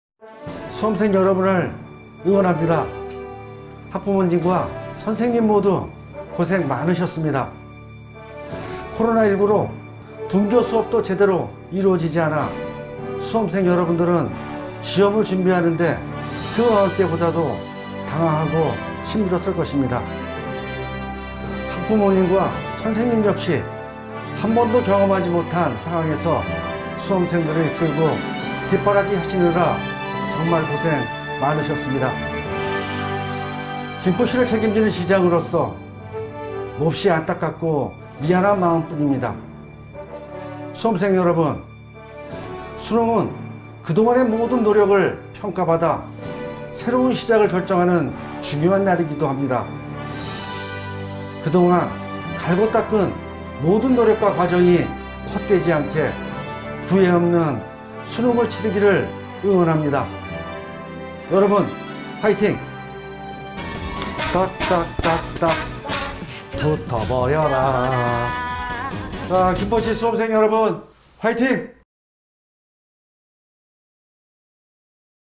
김포수험생여러분 모두 딱! 붙어버려라~! 김포시가 수능대박을 응원합니다
수험생 여러분을 응원합니다!

학부모님과 선생님 모두 고생 많으셨습니다.
코로나19로 등교수업도 제대로 이루어지지 않아
수험생 여러분들은 시험을 준비하는 데 그 어느때보다 당황하고 힘들었을  것입니다.
학부모님과 선생님 역시 한번도 경험하지 못한 상황에서
수험생들을 이끌고 뒷바라지 하느라 정말 고생 많으셨습니다.
김포시를 책임지는 시장으로서 몹시 안타깝고 미안한 마음뿐입니다.
수험생 여러분!
수능은 그동안의 모든 노력을 평가받아 새로운 시작을 결정하는 중요한 날이기도 합니다.
그동안 갈고 닦은 모든 노력과 과정이 헛되지 않게 후회없는 수능을 치르기를 응원합니다.

여러분, 화이팅!

(feat. 딱풀)